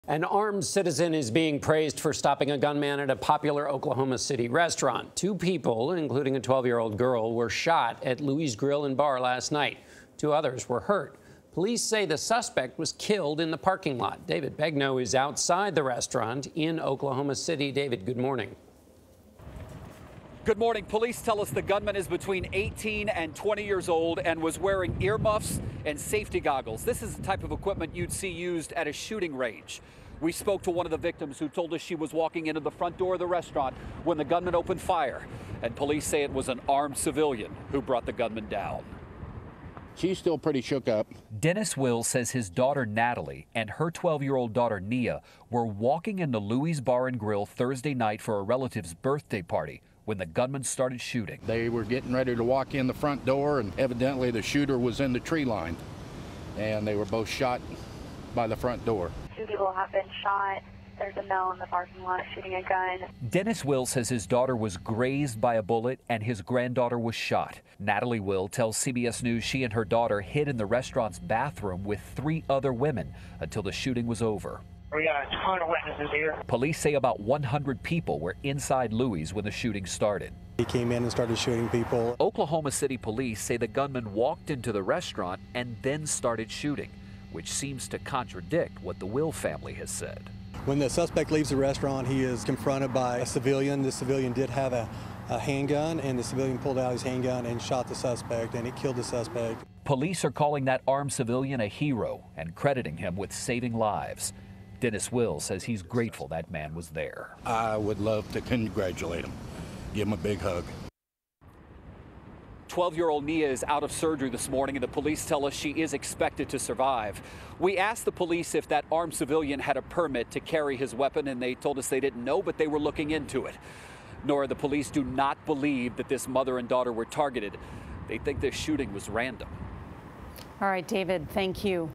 Armed civilian shoots and kills Oklahoma City restaurant gunman
An armed citizen is being praised for stopping a gunman at a popular Oklahoma City restaurant. Two people, including a 12-year-old girl, were shot Thursday night. Police say the suspect was killed in the parking lot. David Begnaud reports.